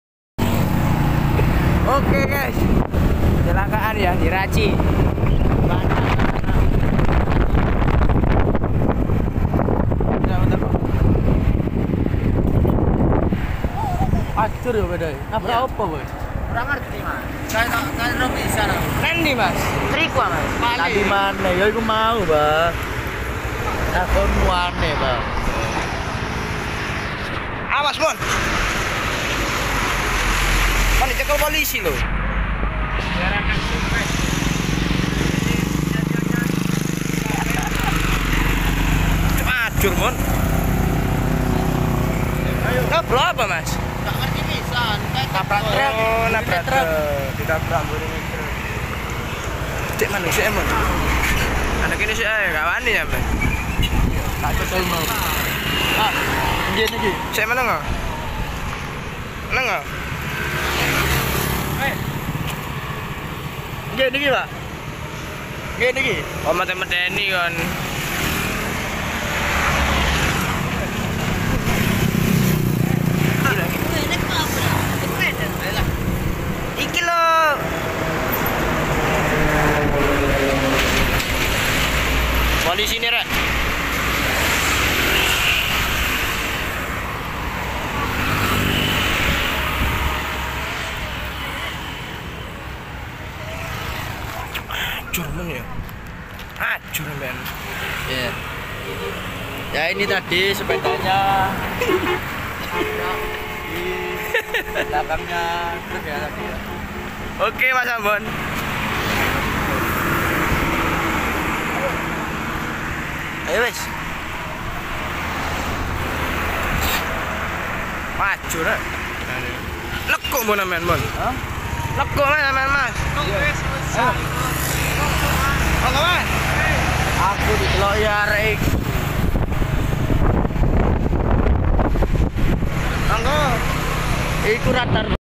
Kecelakaan Di Jalan Raya Raci - Pasuruan
kronologi kata warga setempat menabrak bagian belakang truk
Simak Videonya.

TERIMAKASIH🙏🙏🙏